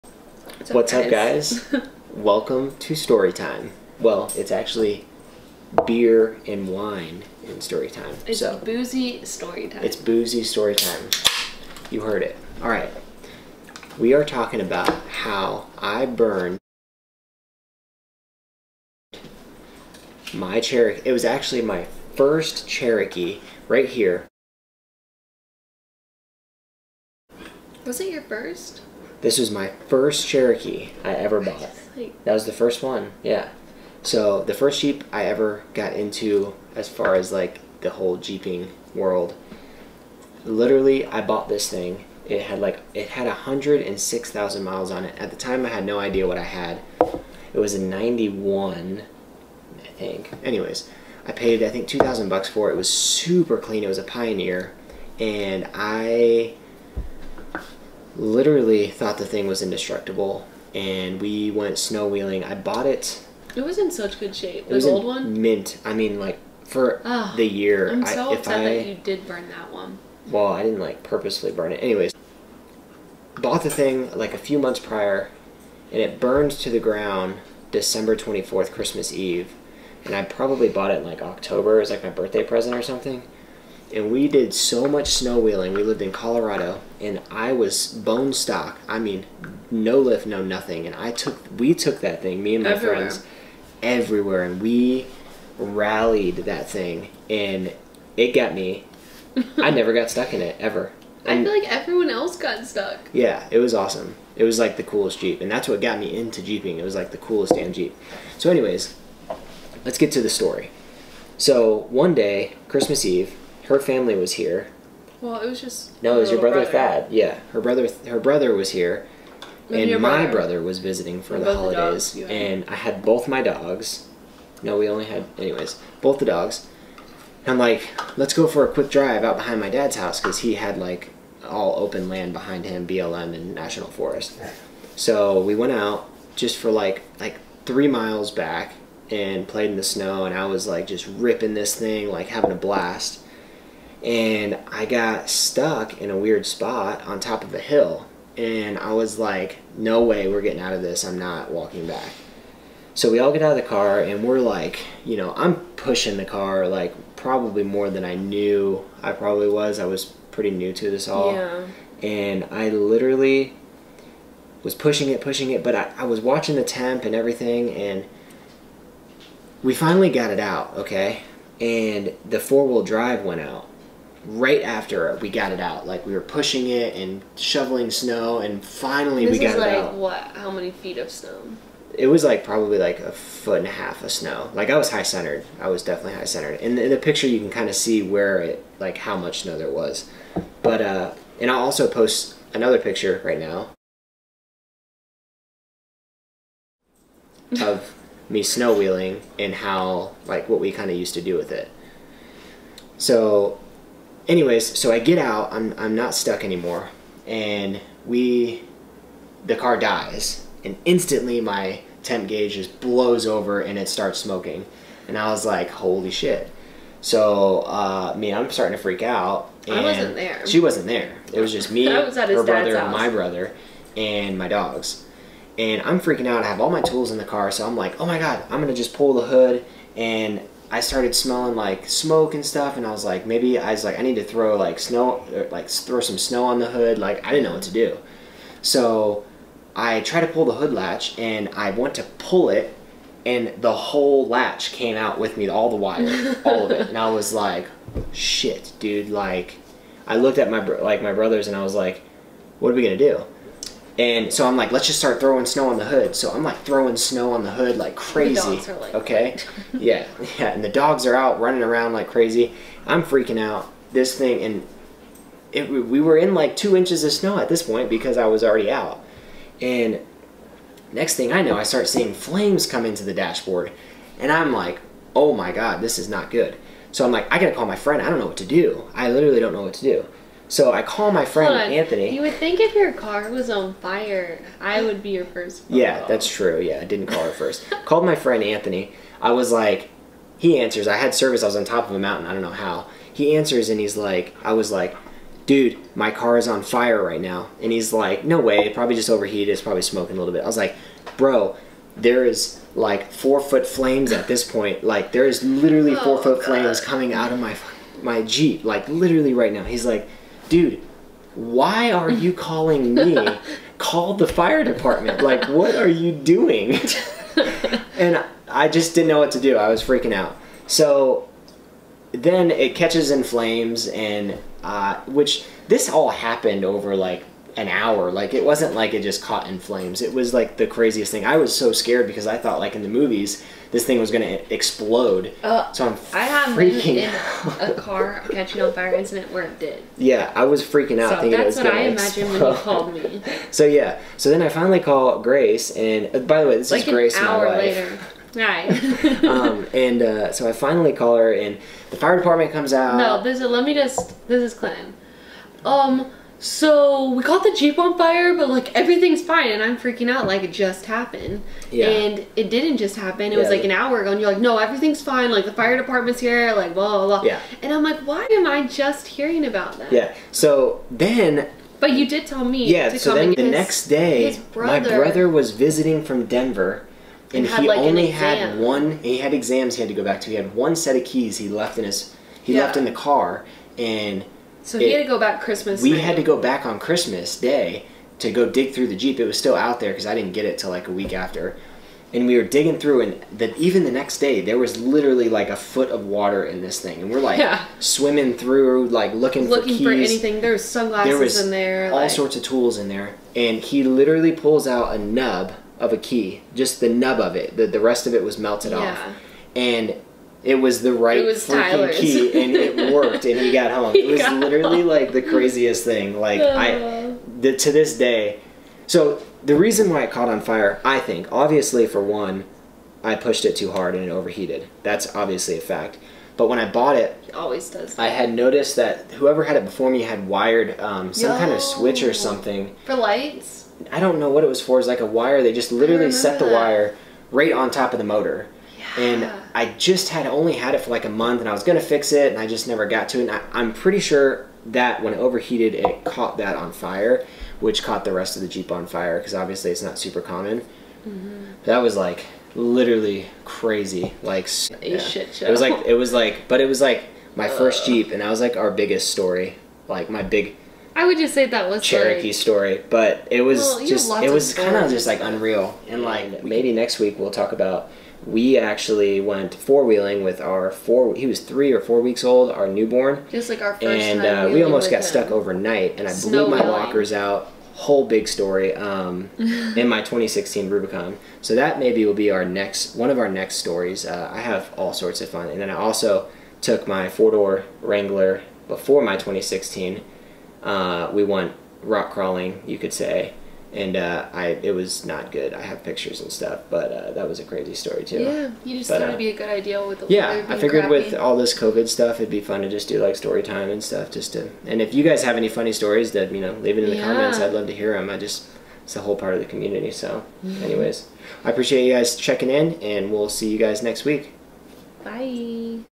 How I burned my Jeep Cherokee XJ | Story-time
This is the full story of how I caught my Jeep Cherokee on fire and it burned to the ground! Hope you enjoy!